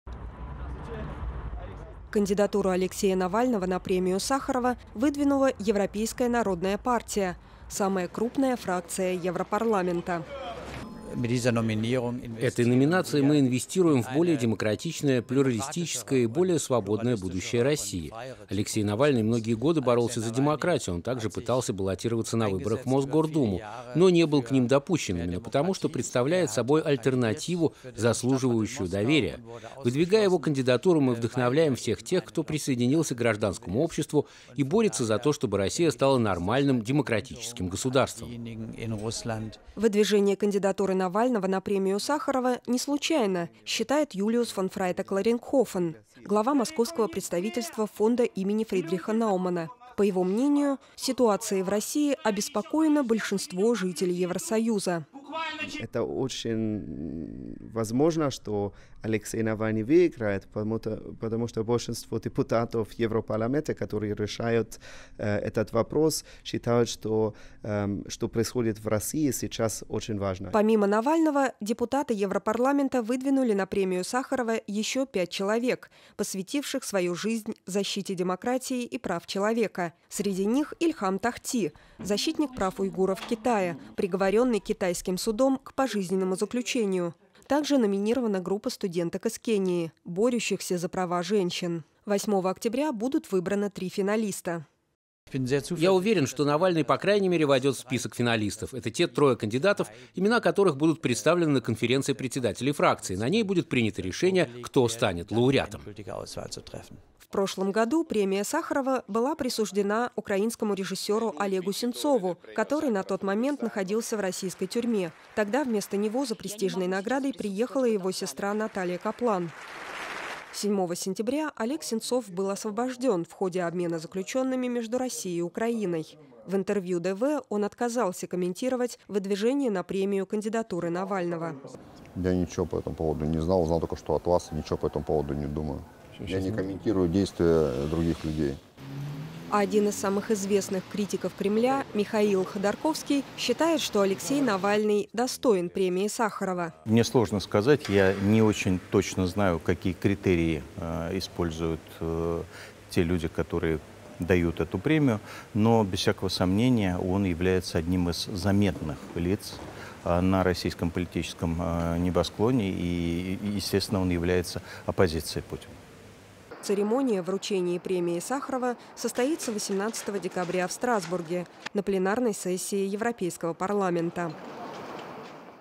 Алексей Навальный номинирован на Премию Сахарова: что думают по этому поводу Сенцов и Ходорковский?
Российский оппозиционер Алексей Навальный номинирован на премию Сахарова, которую вручает Европарламент. Его кандидатуру предложила Европейская народная партия - у нее самая крупная фракция в Европарламенте. В прошлом году премии Сахарова был удостоен украинский режиссер Олег Сенцов, недавно освобожденный в ходе обмена между Москвой и Киевом. Отвечая на вопрос DW, он предпочел не комментировать номинацию Навального. А вот критик Кремля Михаил Ходорковский высказал свое мнение в интервью DW. Церемония вручения премии состоится 18 декабря в Страсбурге. Помимо Алексея Навального на премию выдвинуты еще пять человек. Среди них - Ильхам Тохти, который борется за права уйгуров в Китае.